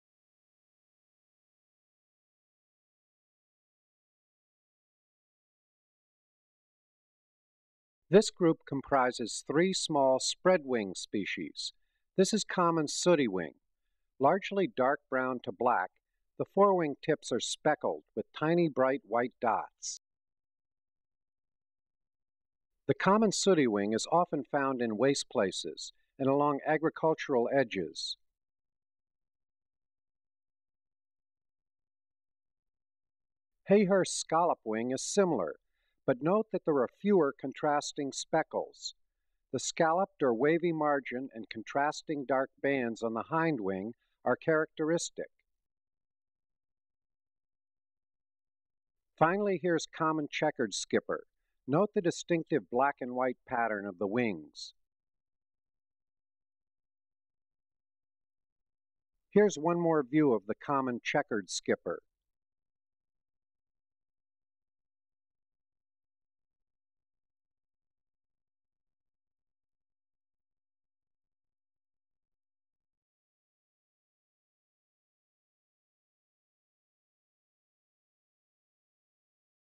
Three Small Spreadwing Skippers
Common Sootywing, Hayhurst's Scollopwing, and Common Checkered-Skipper